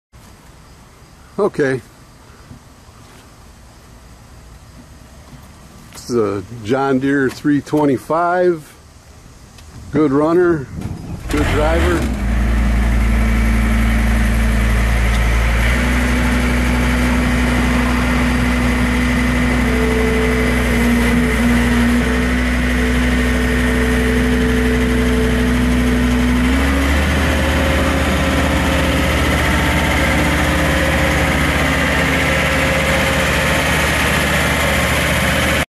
2008 DEERE 325 For Sale
Title: 2008 DEERE 325 For Sale

Description: Cab/Heat, Pilot Controls, 2 Speed, Hyd. Bucket Coupler!Category: Skid Steers